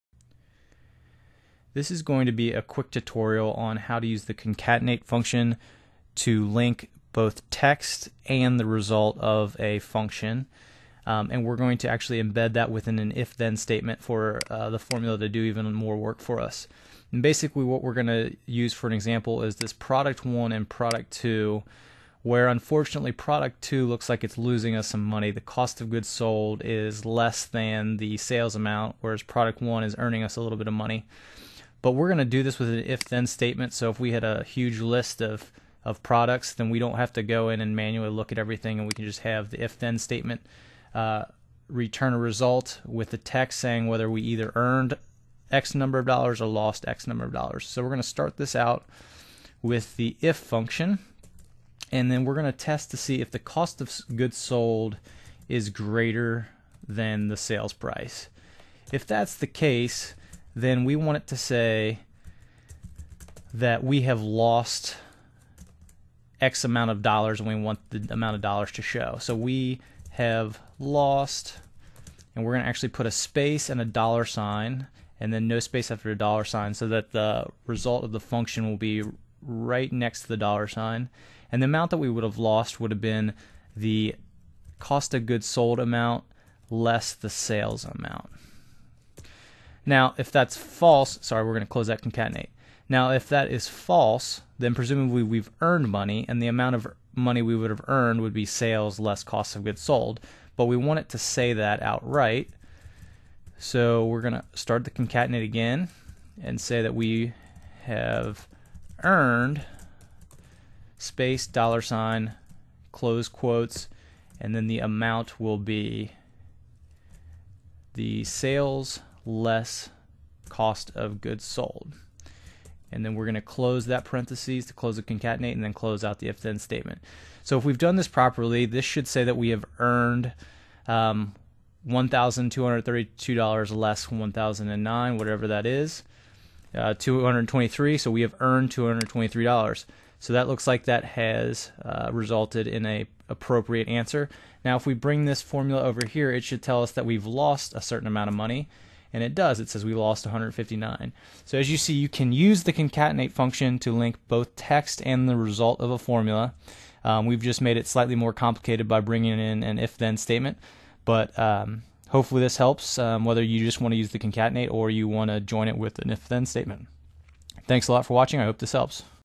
Using Concatenate to link text and a formula
You can make excel "speak" for you with text and the result of a formulat